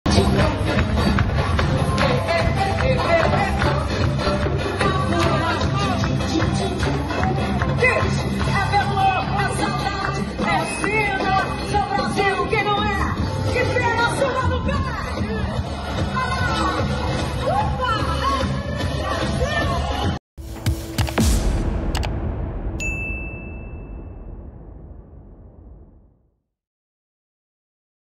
El ridículo baile de Reyes Maroto junto a dos bailarines en un mitin con menos de 30 personas
Lo dijo en su día el president de la Generalitat Josep Tarradellas:»En política se puede hacer de todo menos el ridículo.» Y la alcaldable del PSOE en Madrid, Reyes Maroto, lo ha hecho y de qué manera durante un mitin multicultural celebrado en el Auditorio al aire libre Cerro Almodóvar de la capital española. La exministra de Industria, Comercio y Turismo, con aquello de que en campaña vale todo, se animó a bailar junto a dos bailarines descamisados… a los que le costó seguir el ritmo.